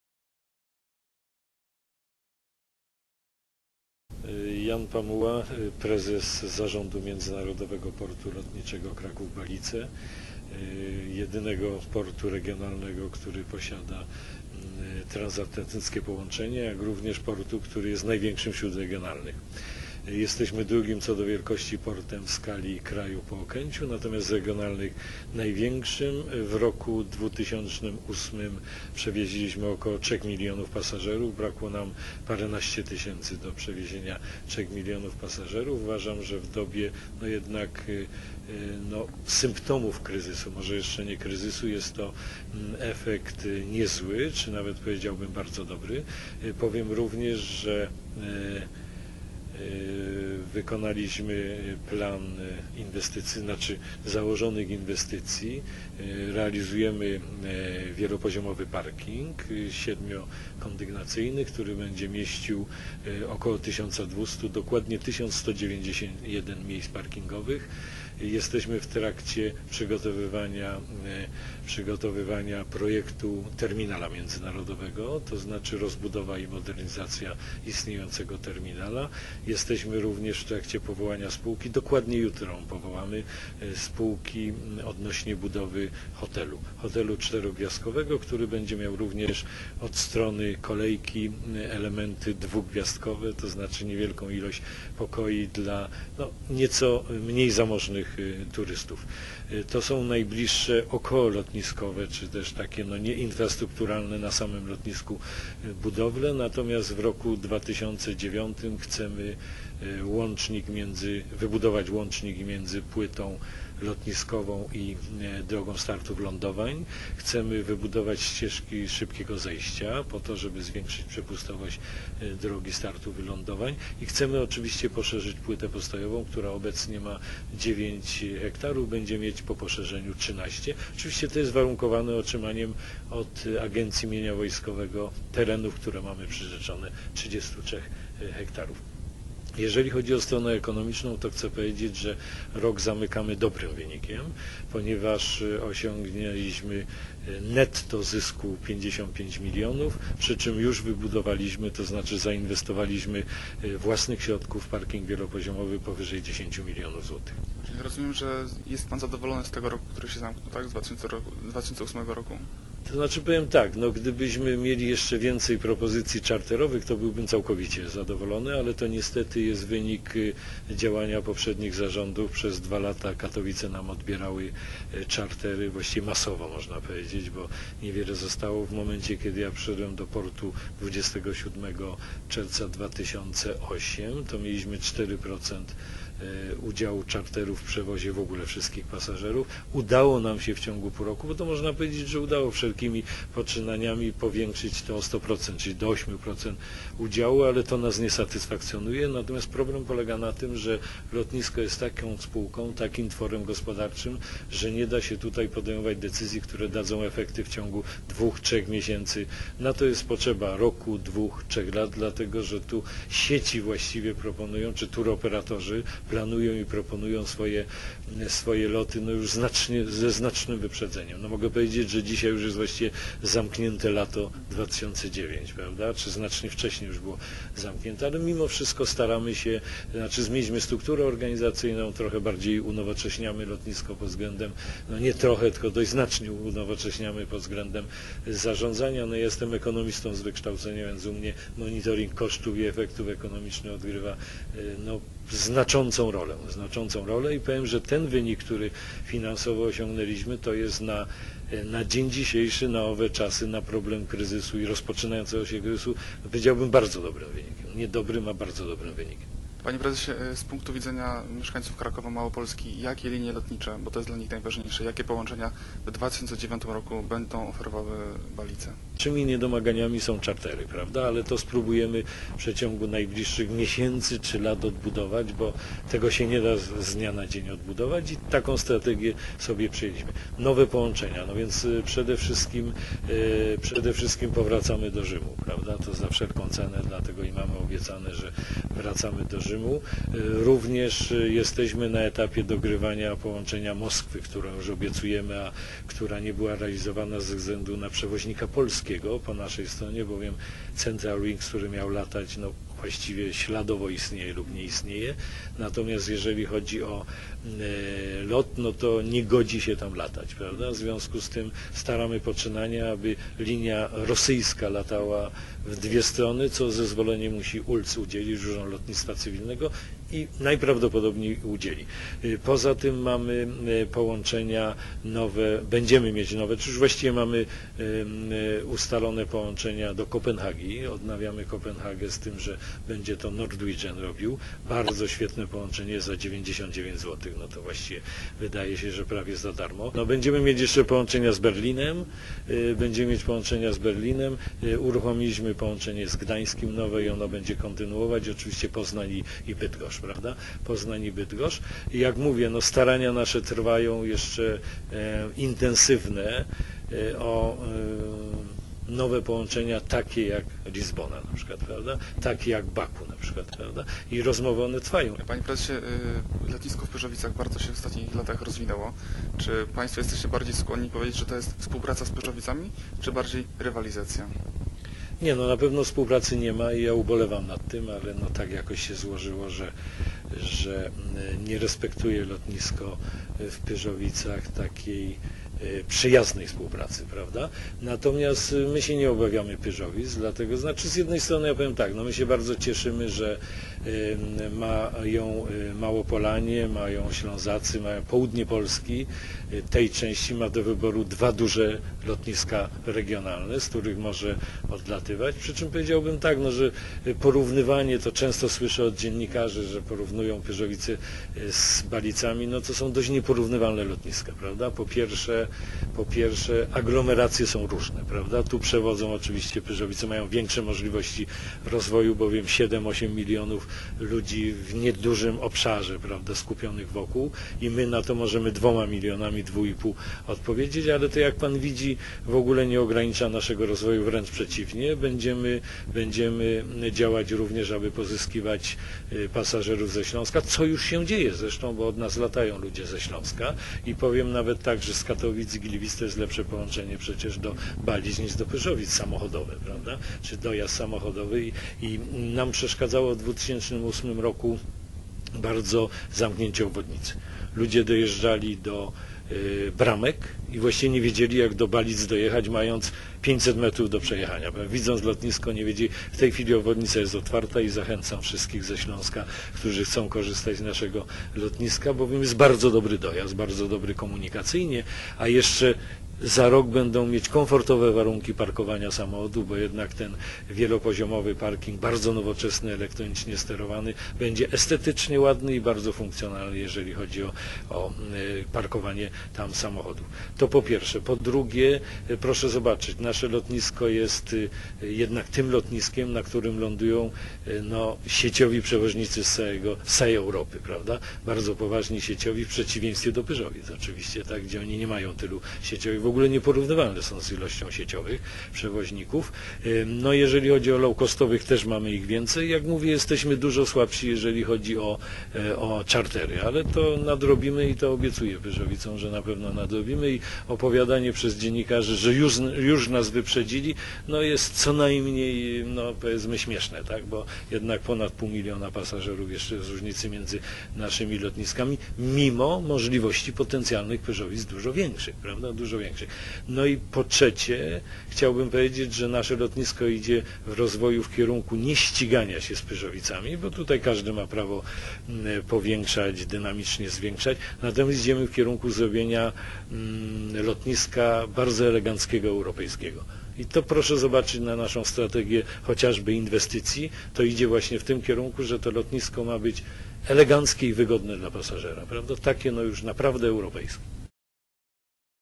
Newsroom jest wszędzie - lotnisko w Balicach
Byliśmy tam i rozmawialiśmy o kondycji lotniska.